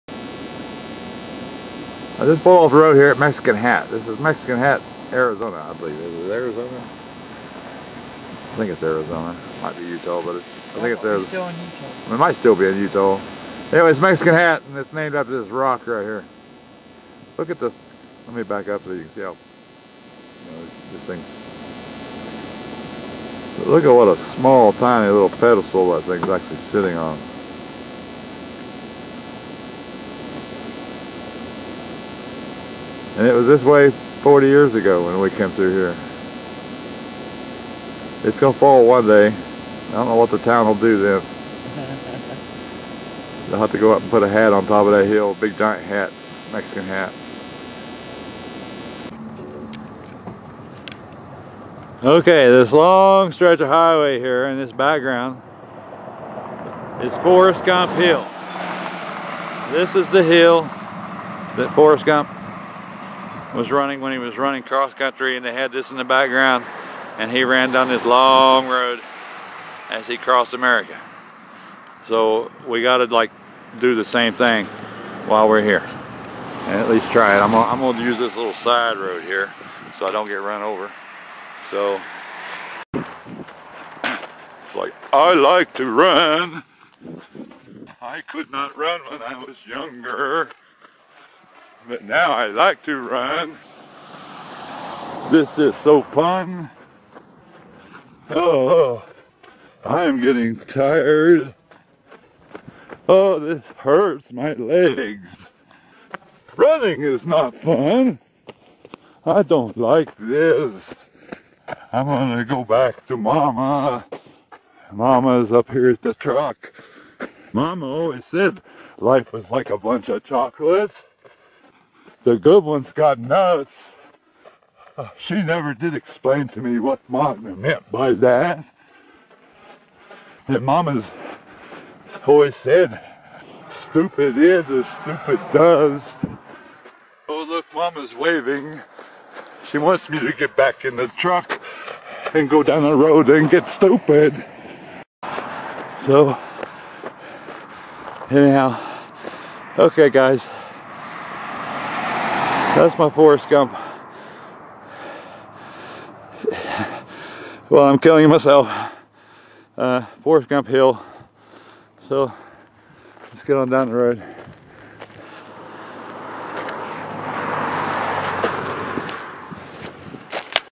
MEXICAN HAT FORREST GUMP HILL
First Mexican Hat Rock Then A visit to Forrest Gump Hill